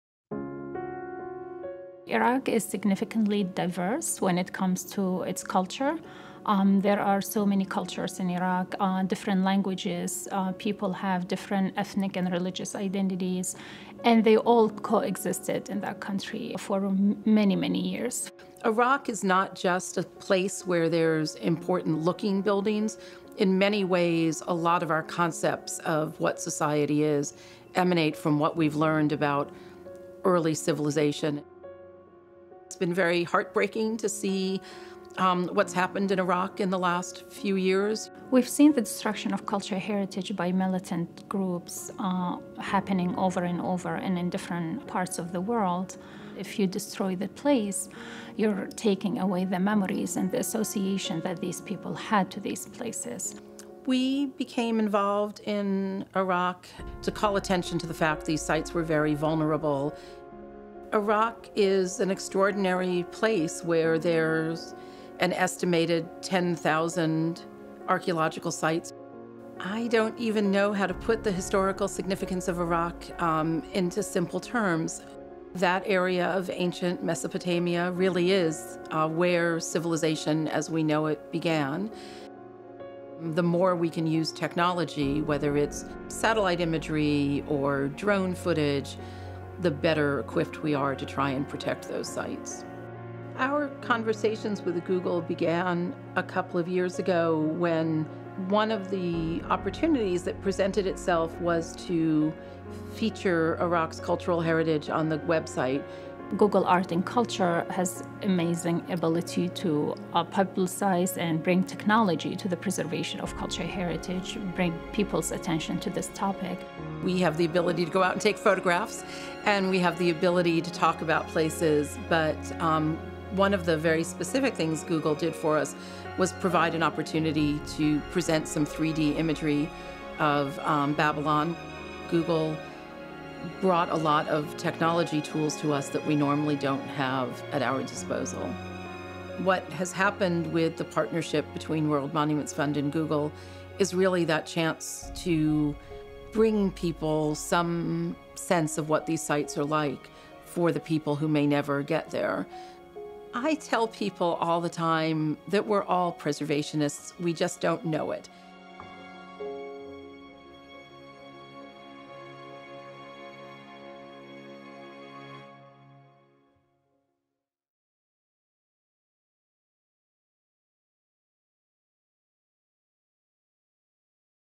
Preserving Iraq's Heritage with World Monuments Fund | Google Arts & Culture
The  World Monuments Fund’s “Preserving Iraq’s Heritage” project showcases the efforts towards preserving Iraq´s cultural heritage amid human conflict and war.